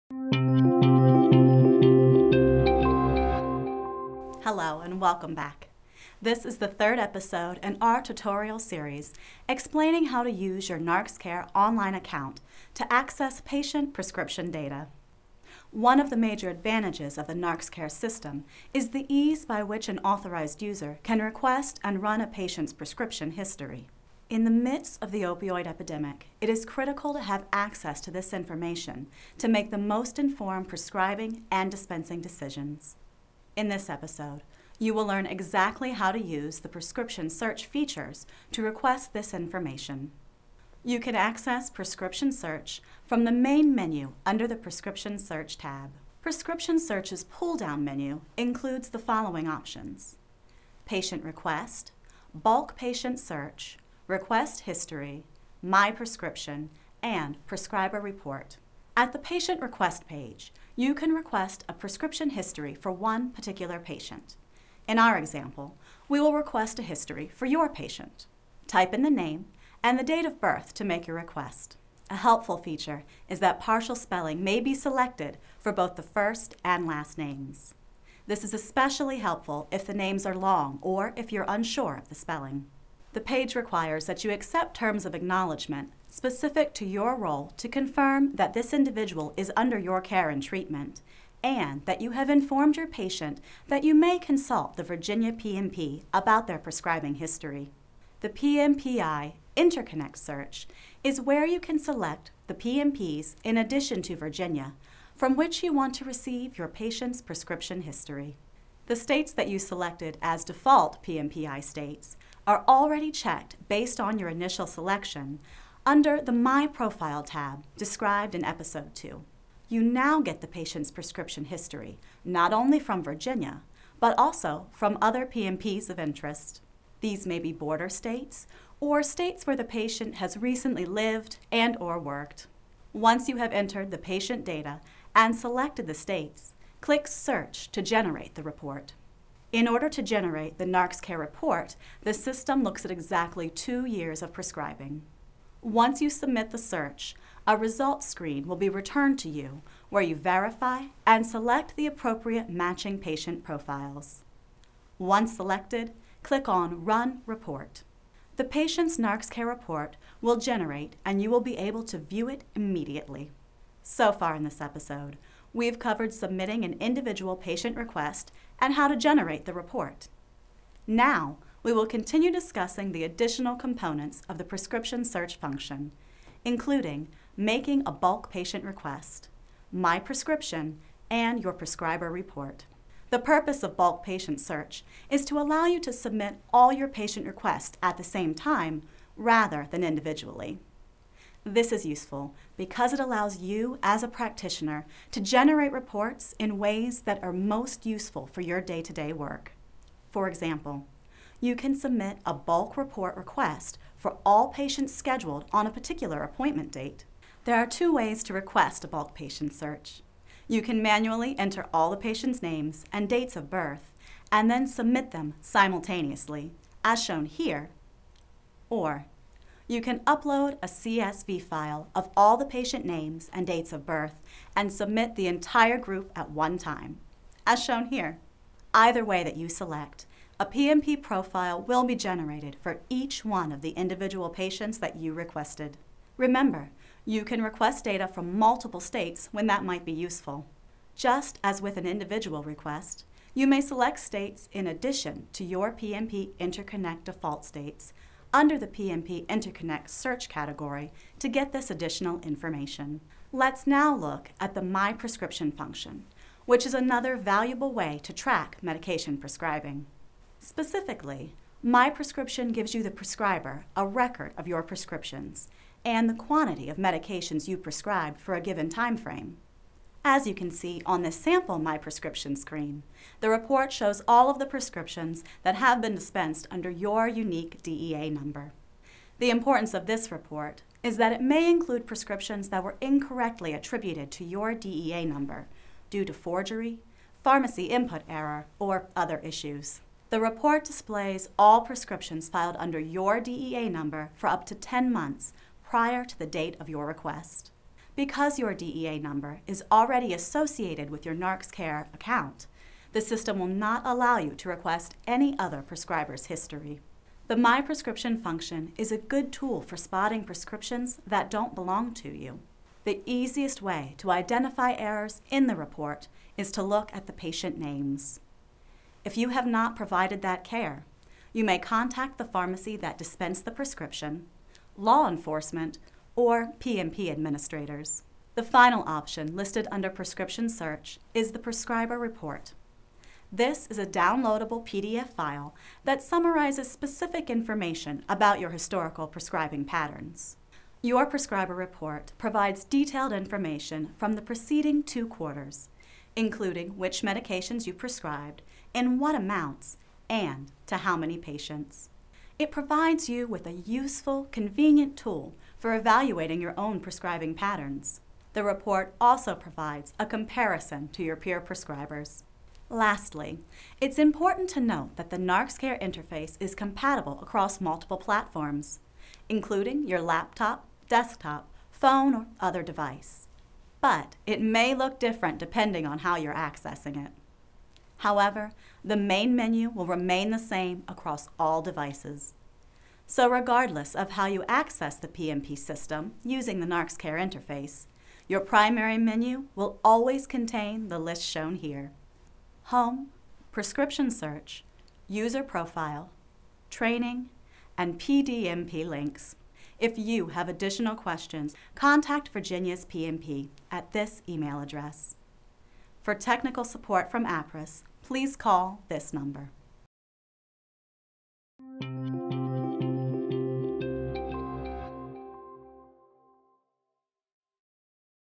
Navigating NarxCare - Rx Search
Navigating NarxCare is a video series intended for registered users of Virginia's PMP.  Healthcare practitioners can learn how to use and interpret the software securely and efficiently.